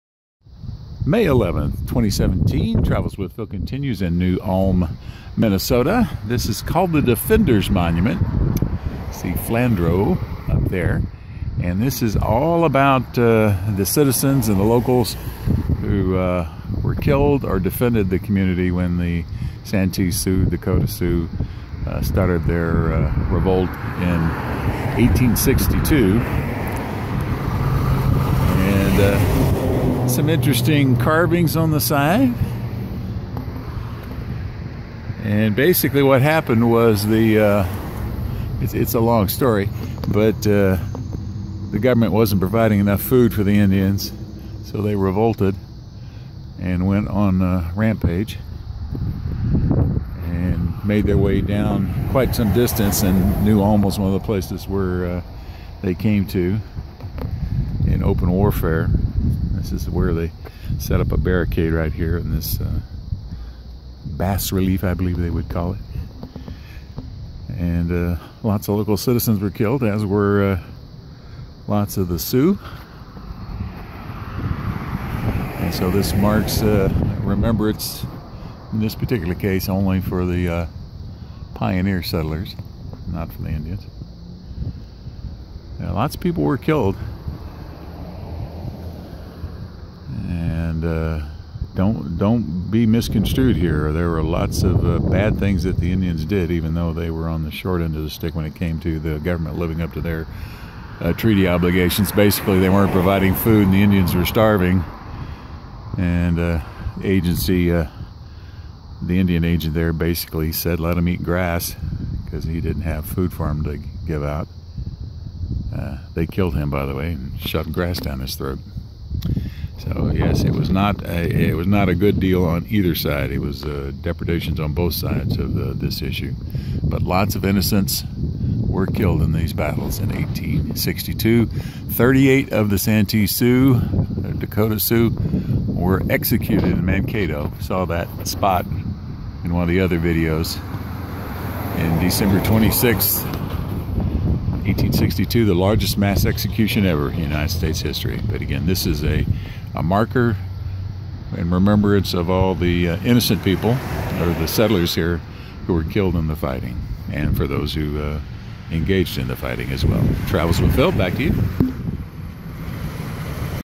New Ulm Defenders Monument, Minnesota - May 11, 2017 - Travels With Phil - Unedited
The New Ulm Defenders Monument in Minnesota. It is dedicated to the settlers and soldiers who fought in the war of 1862 against the Dakota Sioux. - . Travels with Phil by Phil Konstantin -